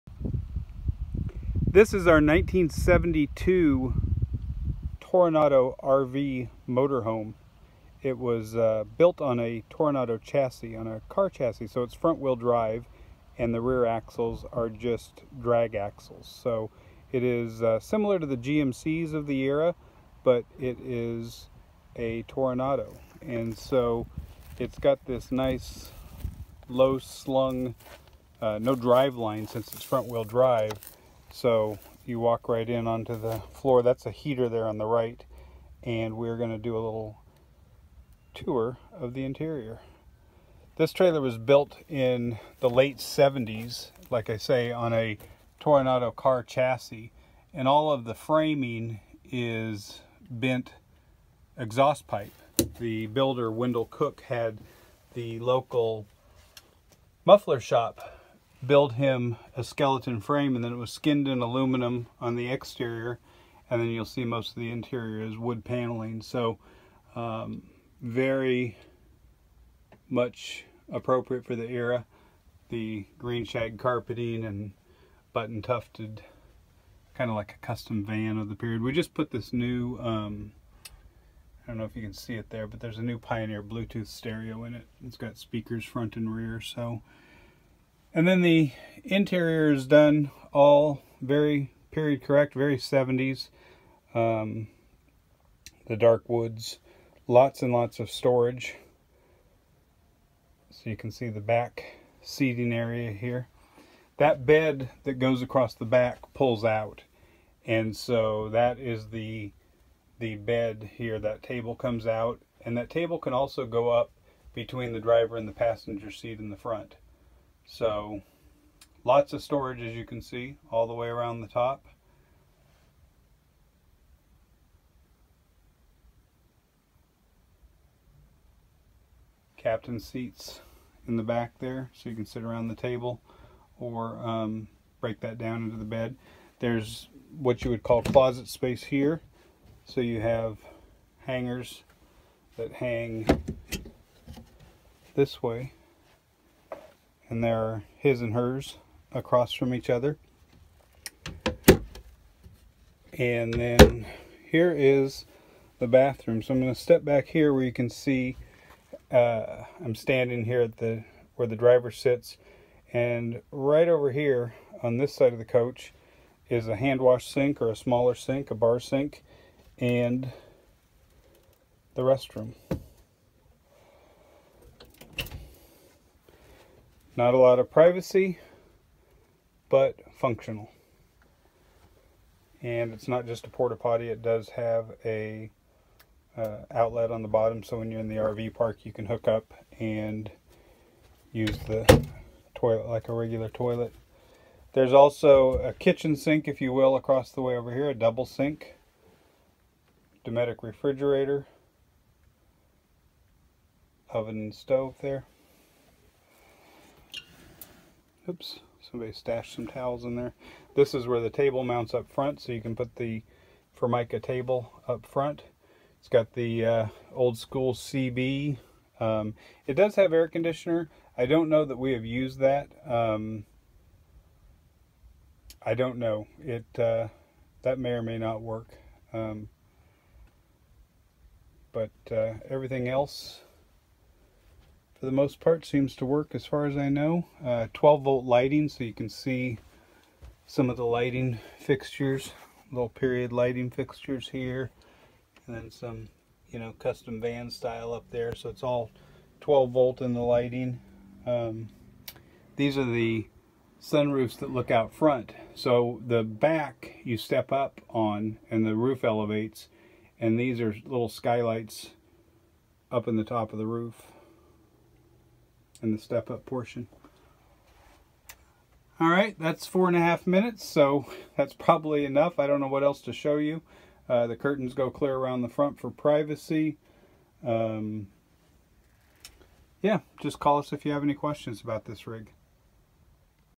Custom Built, one-of-a-kind, Vintage RV
Built in the late 1970s on a 1972 olds Toronado chassis.